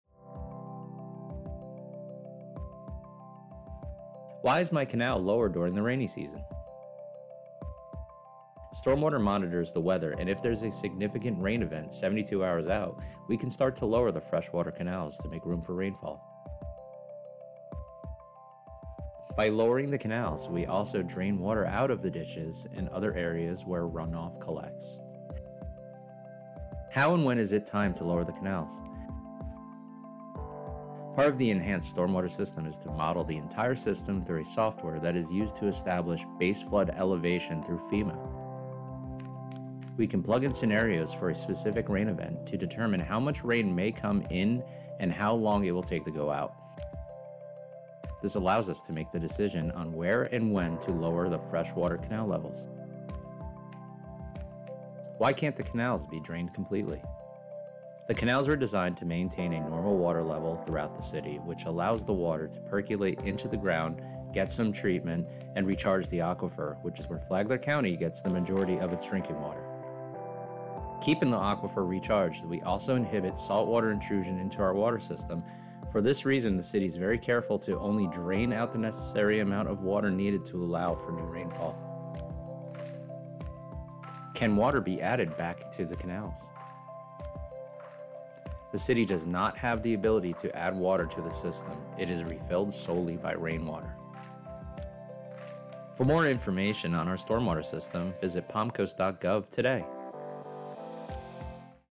Stormwater 101: Canal Levels
Learn about important information on the City of Palm Coast's Stormwater System and how city staff has the ability to monitor and control water levels in the city's freshwater canals.

Did you know that the City's Stormwater & Engineering Department can proactively lower the water levels in our freshwater canals throughout the city? Lowering the canal water levels increases the capacity to handle excessive rainfall.